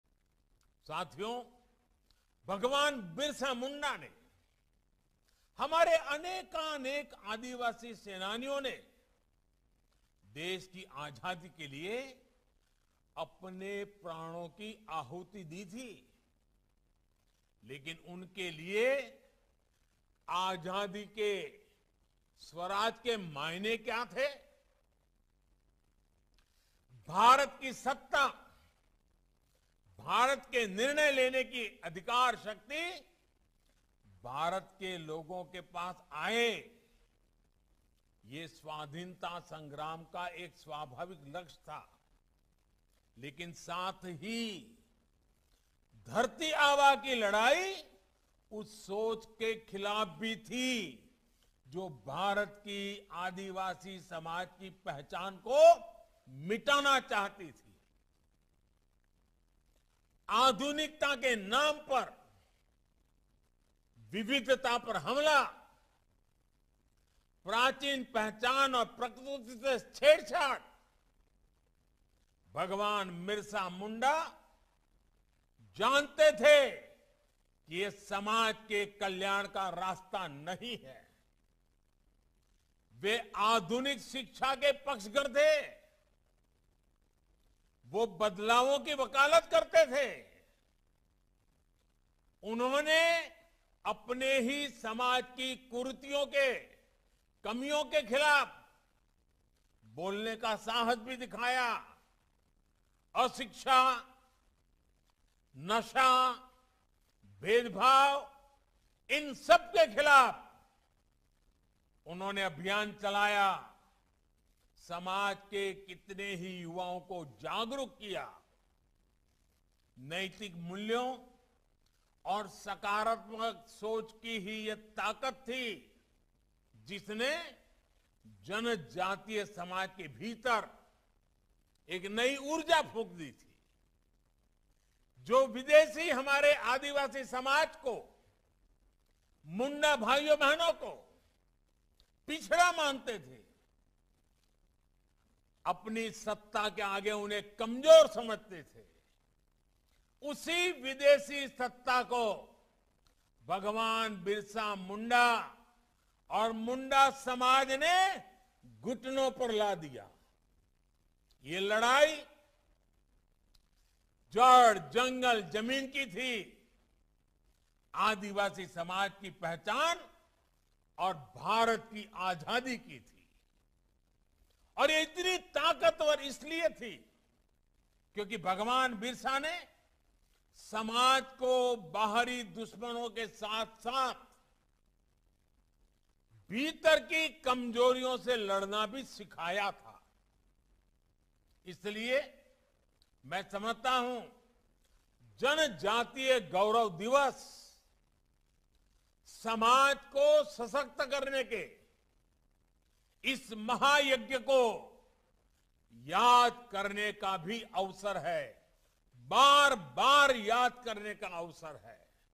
Bhagwan Birsa Munda fought against the evils in the society: PM
Government of India has declared that the birth anniversary of Bhagwan Birsa Munda will be celebrated as Janjatiya Gaurav Diwas. To mark the occasion, Prime Minister Shri Narendra Modi inaugurated Bhagwan Birsa Munda Memorial Udyan cum Freedom Fighter Museum - Bhagwaan Birsa Munda Museum at Ranchi today via video conferencing. Governor and Chief Minister of Jharkhand and Union Ministers were among those present on the occasion.